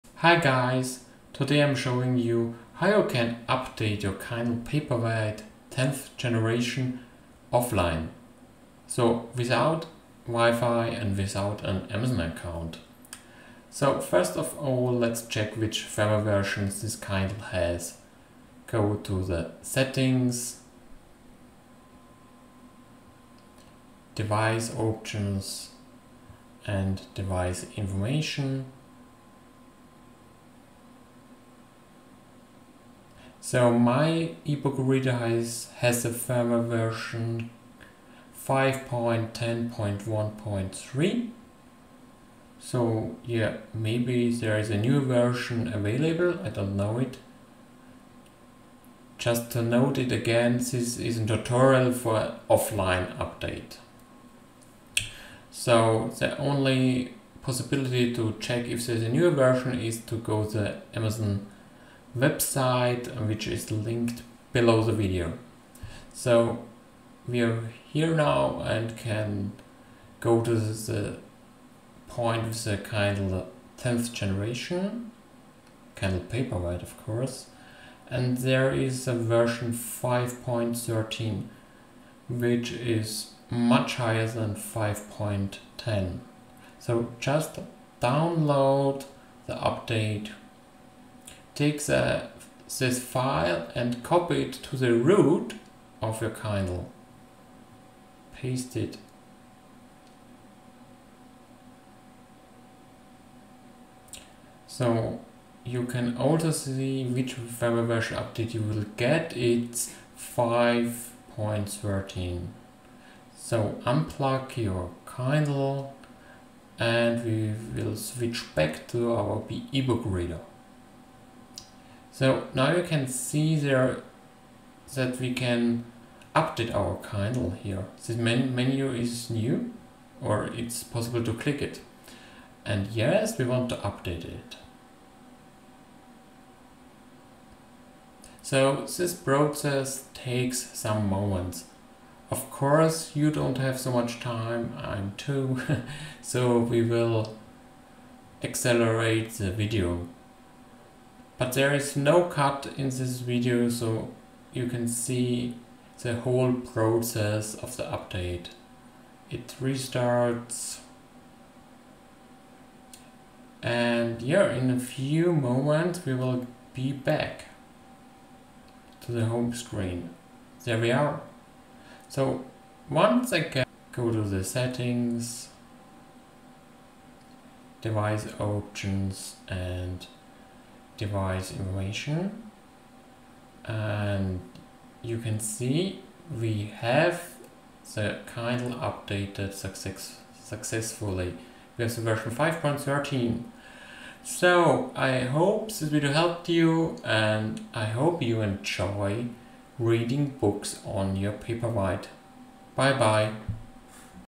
Kindle Paperwhite Offline Firmware Update [English]
A short tutorial how to update your Paperwhite offline and without an amazon account.

Firmware download